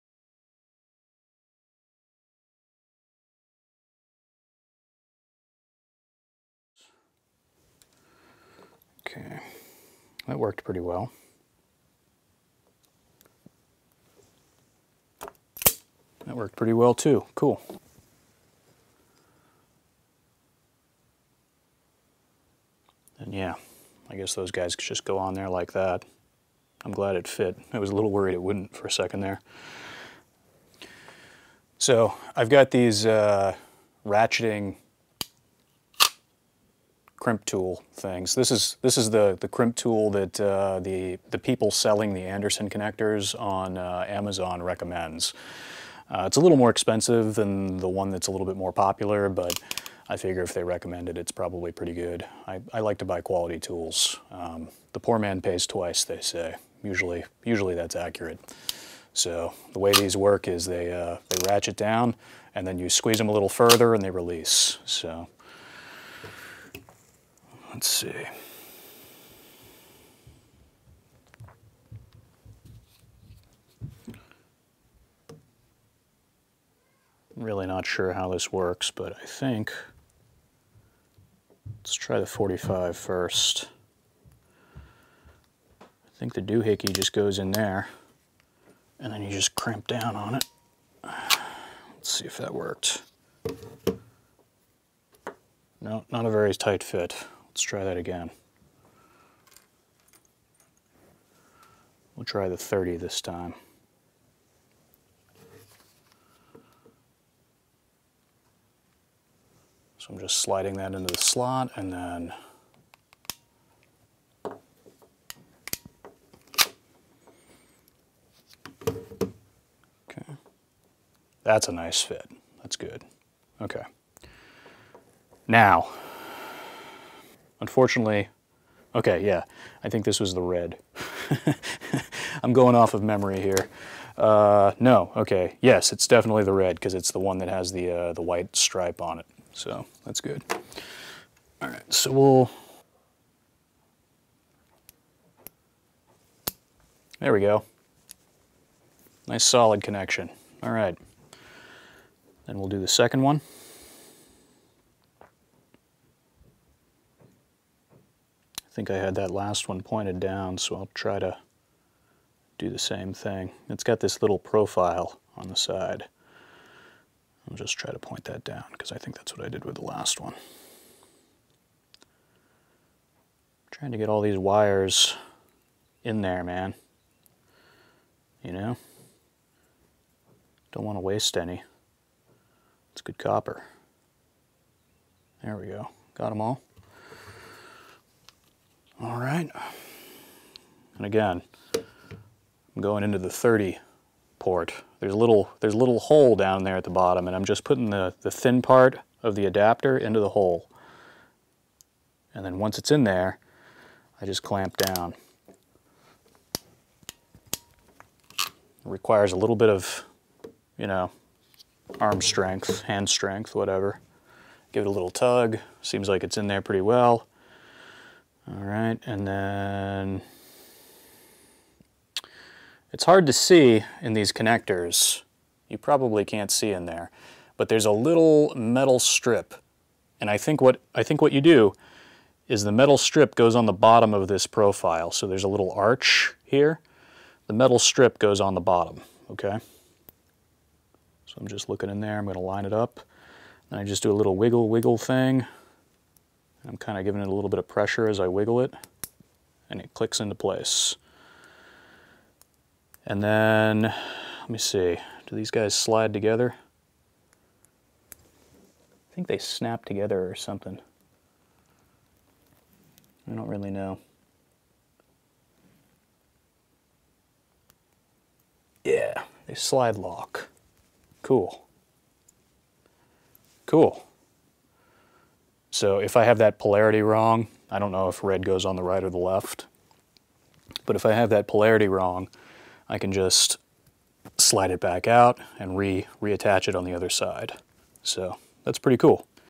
How to Crimp Anderson Connectors
Anderson connectors are often used in 12V systems, like cars, trucks, or even solar generators. I use mine with my Goal Zero Yeti 400. They're pretty easy to work with.

I bought all of these tools and supplies on Amazon. Click the links below if you're interested in these too.